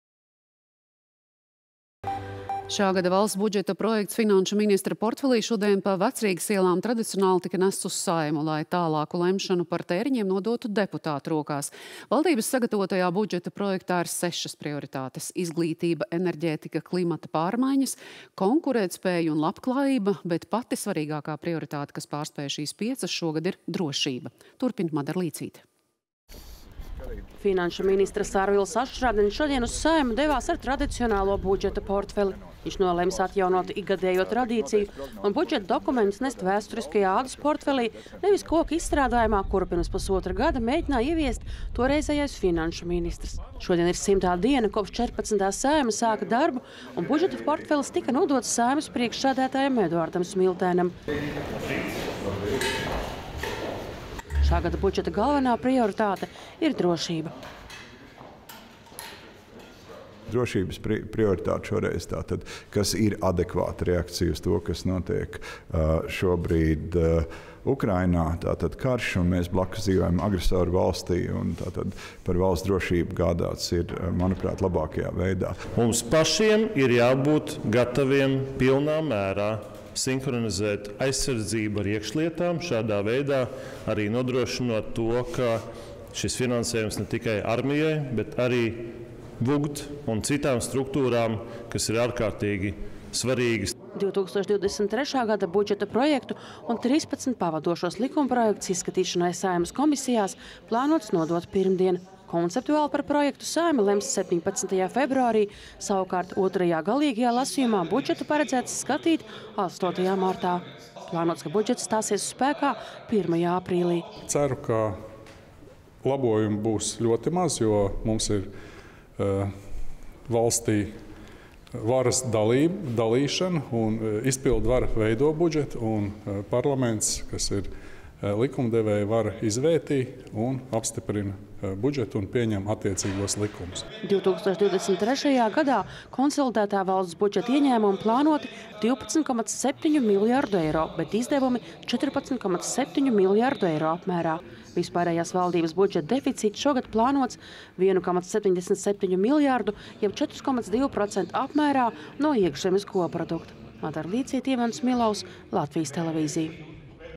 Saeimā iesniedz šā gada budžeta projektu
Šā gada valsts budžeta projekts finanšu ministra portfelī šodien pa Vecrīgas ielām tradicionāli tika nests uz Saeimu, lai tālāku lemšanu par tēriņiem nodotu deputātu rokās. Valdības sagatavotajā budžeta projektā ir sešas prioritātes – izglītība, enerģētika, klimata pārmaiņas, konkurētspēja un labklājība. Bet pati svarīgākā prioritāte, kas pārspēj šīs piecas, šogad ir drošība. Turpina Madara Līcīte.